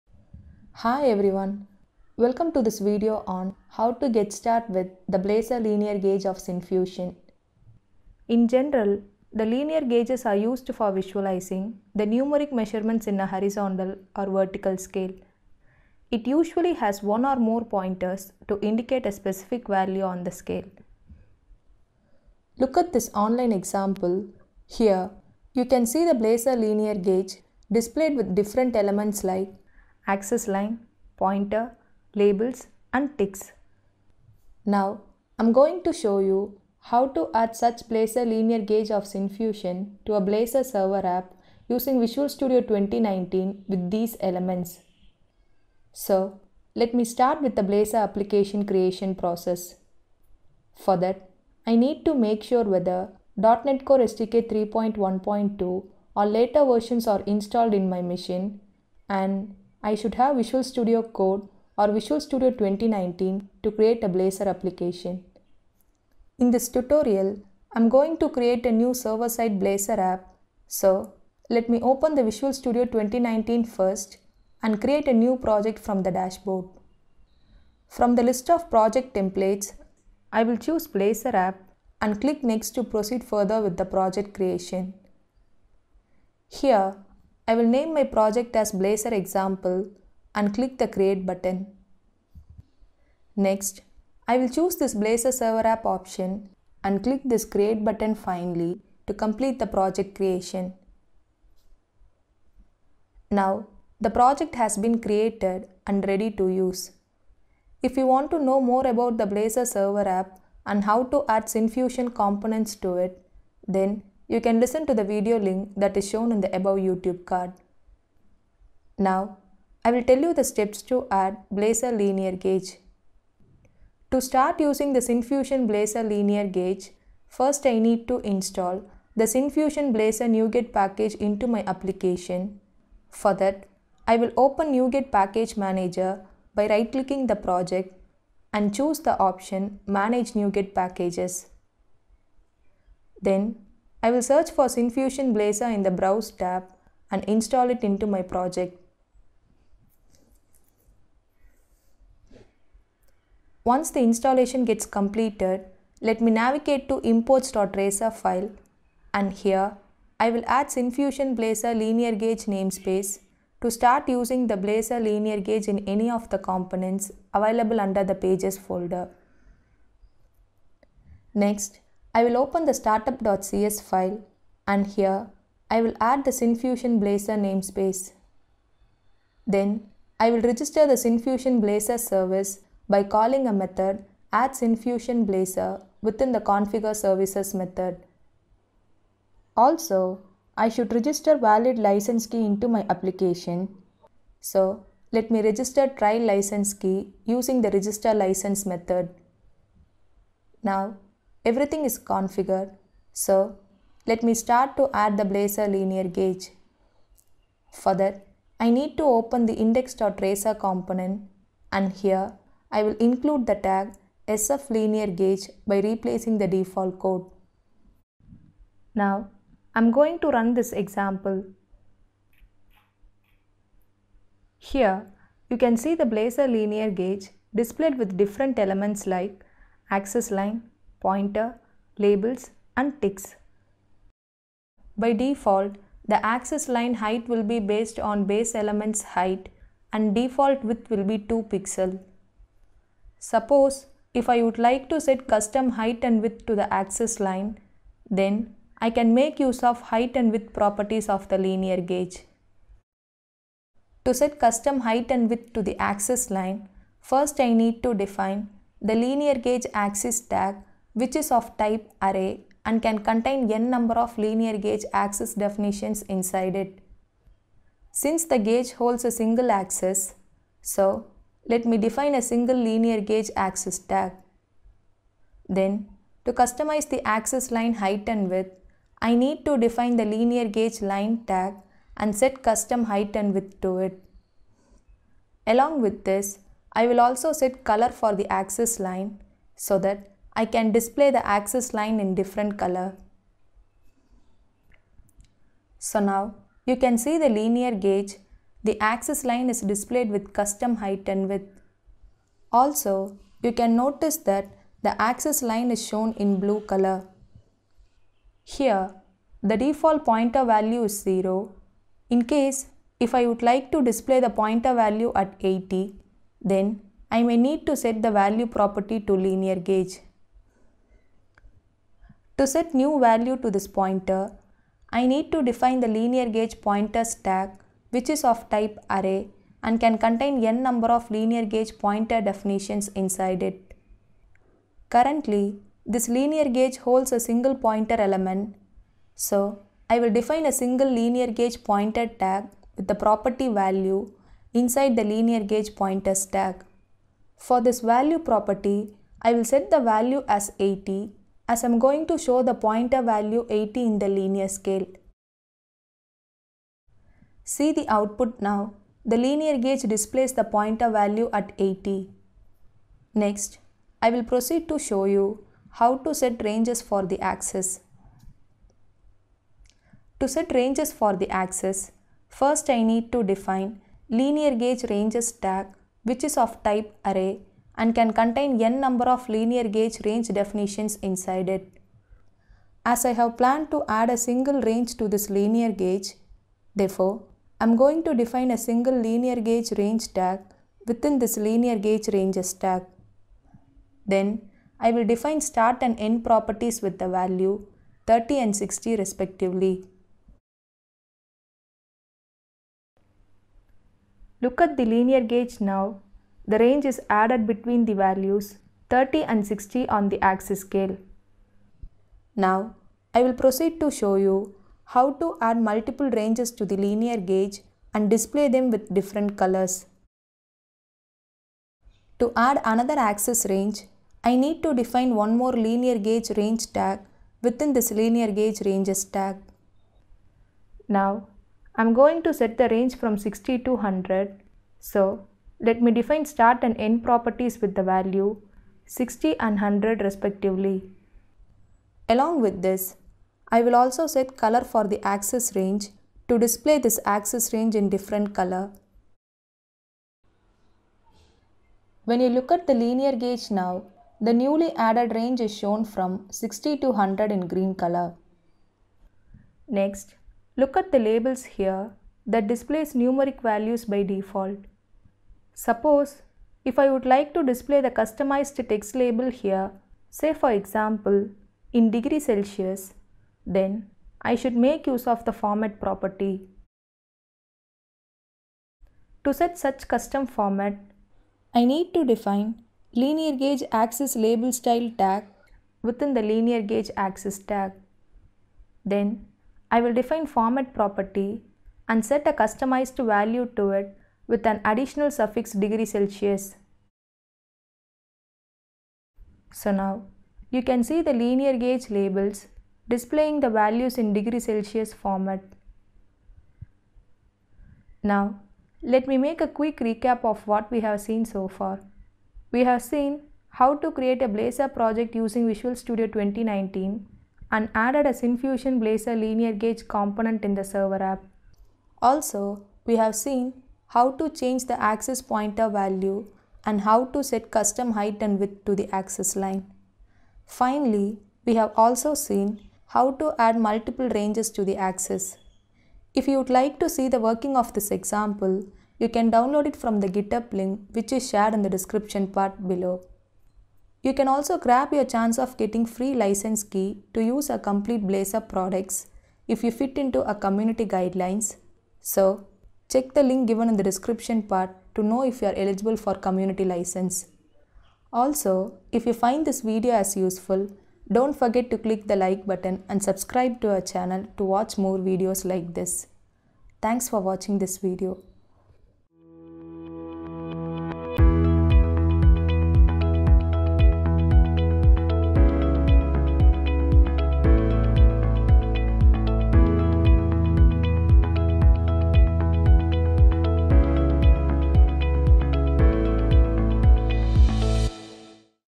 Add Syncfusion Blazor Linear Gauge to Blazor Server App
A quick overview on how to create and configure the Syncfusion Blazor Linear Gauge in a Blazor Server app using Visual Studio 2019.

In this video, you will learn how to add a simple Blazor Linear Gauge to a Blazor Server app. You will also see how to set a specific axis pointer value as well as how to customize an axis line height and width. Finally, you will learn how to add multiple axis ranges.

TRIAL LICENSE KEY:
If you need a trial license key, start your Blazor trial in your Syncfusion account, and then get your trial license key from the Downloads page.

BOOKMARK DETAILS
[01:11] Create a Blazor Server app
[02:40] Add a Blazor Linear Gauge
[04:14] Customize an axis line
[05:23] Change pointer values
[06:10] Add multiple axis ranges
[07:59] Set custom label format